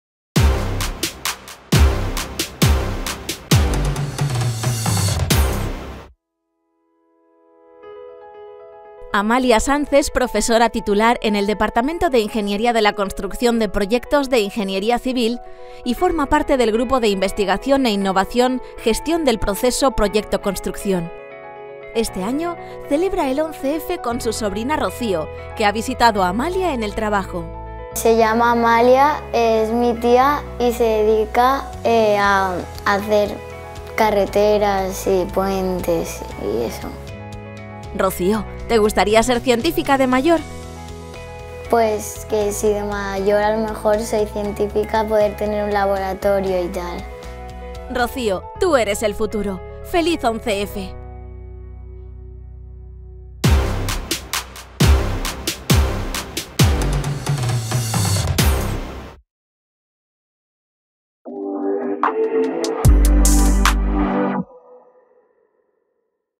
11F 2024 - Amalia Sanz y Rocío
En la Universitat Politècnica de València celebramos este año el Día de la Mujer y la Niña en la Ciencia llevando a futuras investigadoras a los laboratorios.

Amalia Sanz es profesora Titular en el Departamento de Ingeniería de la Construcción y de Proyectos de Ingeniería Civil, y forma parte del grupo de investigación e innovación Gestión del Proceso Proyecto-Construcción.

Este año celebra el 11 F con su sobrina Rocío, que ha visitado a Amalia en el trabajo.